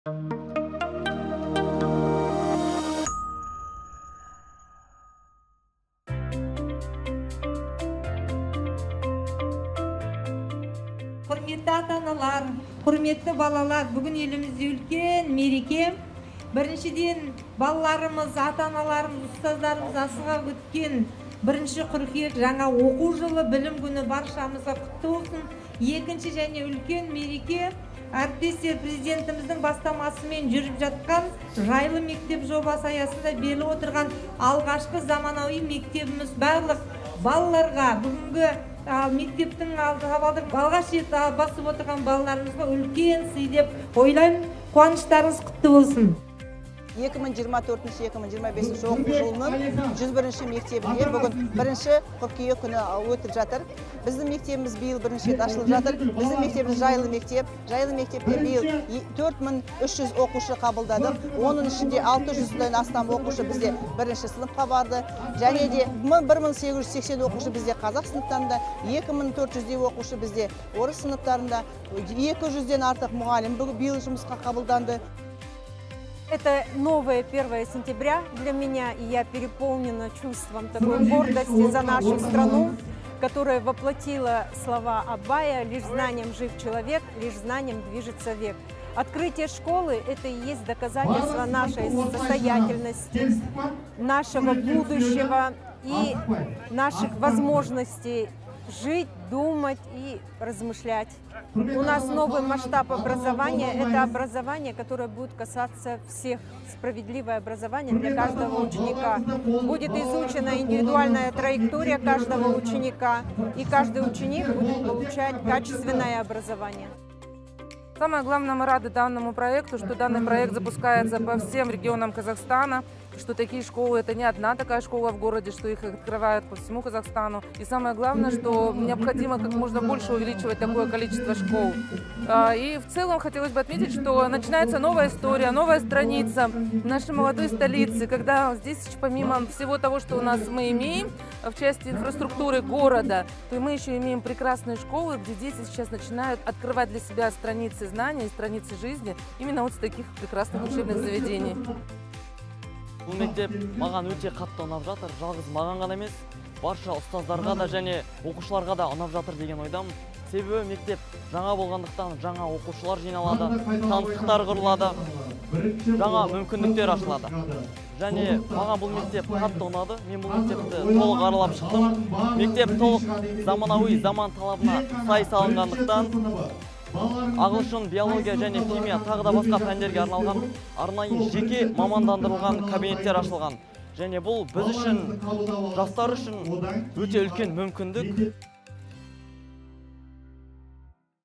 ҰЛТТЫҚ ЖОБА АЯСЫНДА ЕЛОРДАДА ЖАҢА «ЖАЙЛЫ МЕКТЕП» АШЫЛДЫ
№101 орта мектеп директорының оқу жұмысы жөніндегі орынбасары Айбарша Ещанова:
«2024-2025 оқу жылының 101-ші мектебінде бүгін 1-ші қыркүйек күні өтіп жатыр. Біздің мектебіміз биыл бірінші рет ашылып жатыр. Біздің мектебіміз – «Жайлы мектеп». Жайлы мектепке биыл 4300 оқушы қабылдадық. Оның ішінде 600-ден астамы бірінші сыныпқа барды. 1880 оқушы қазақ сыныптарында, 2400-дей оқушы орыс сыныптарында оқиды. 200-ден астам мұғалім жұмысқа қабылданды».